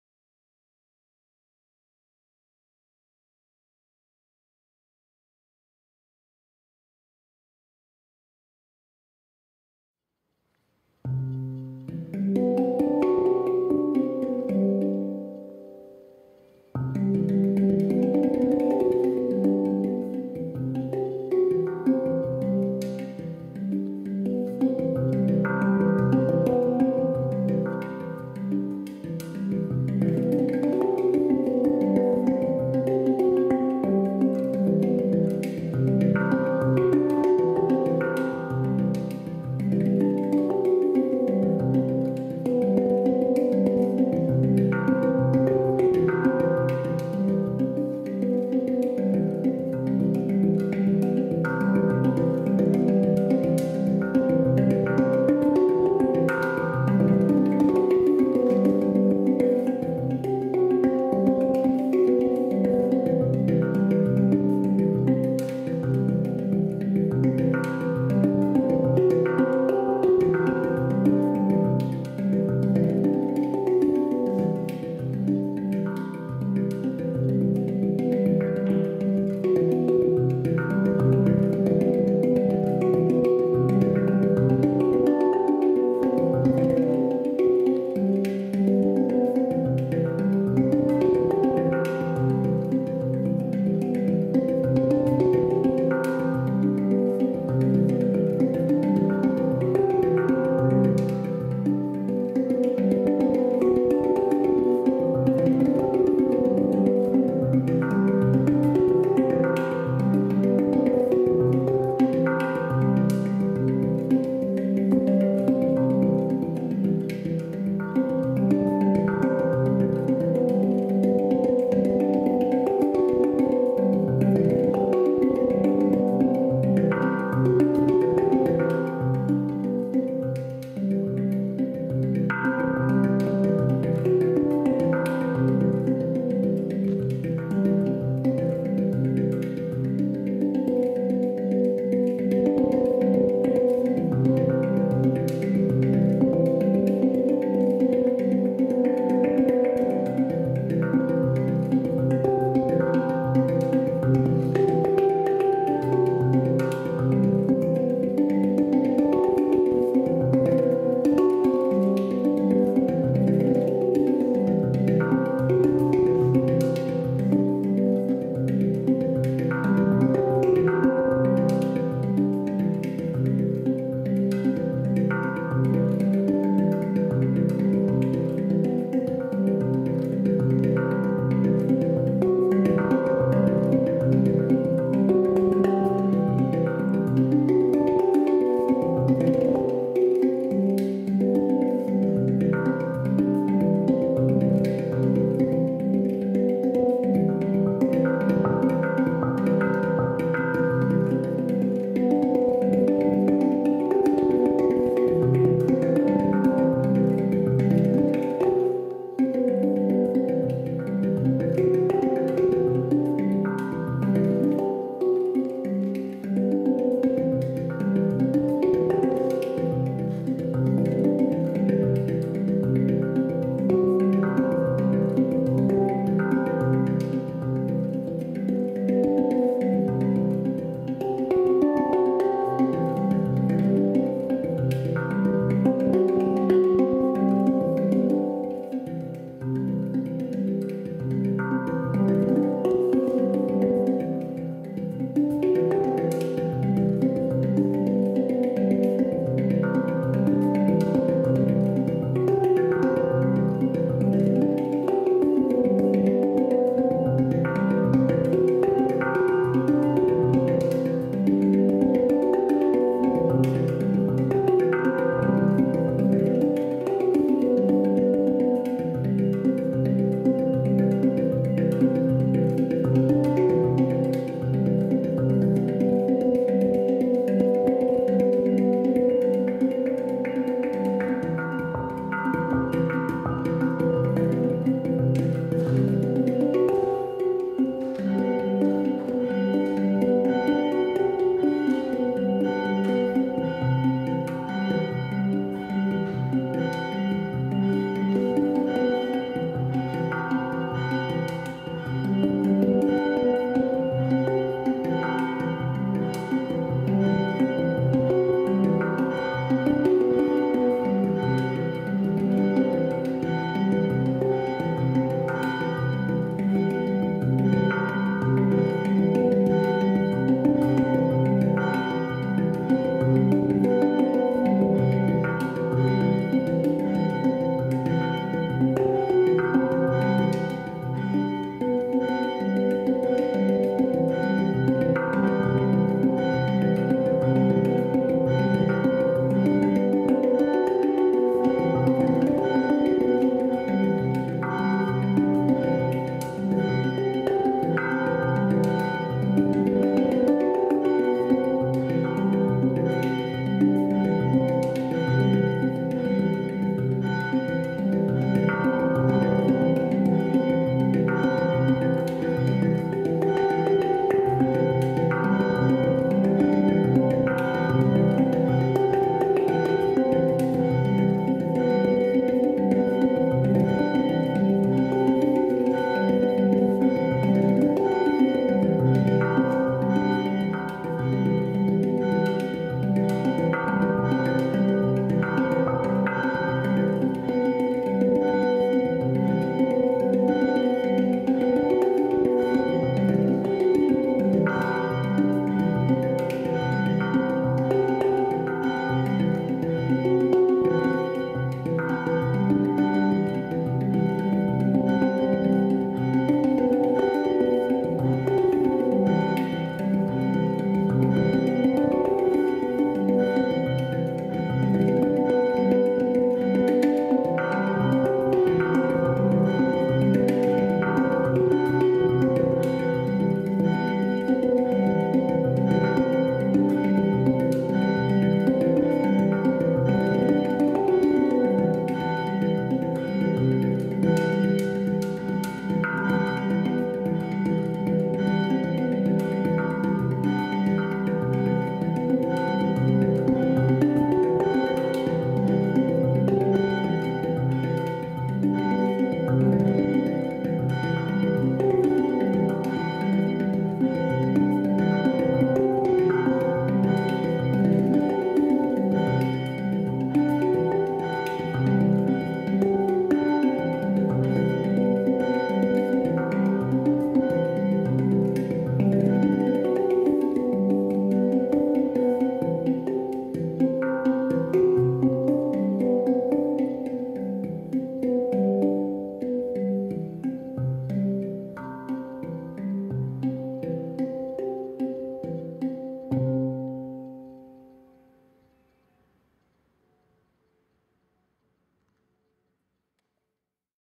Halo Golden Gate - erste Gehversuche
Heute ist mein Halo eingetroffen. Hier ein kleiner Film mit allem Zubehör, das mitgeliefert wird. Der Klang ist prima und das als Sonderzubehör erhältliche Hardcase riesig und sehr gut verarbeitet. Zum Zubehör gehören Pflegetücher und ein Säckchen mit Granulat zur Entfernung von Feuchtigkeit.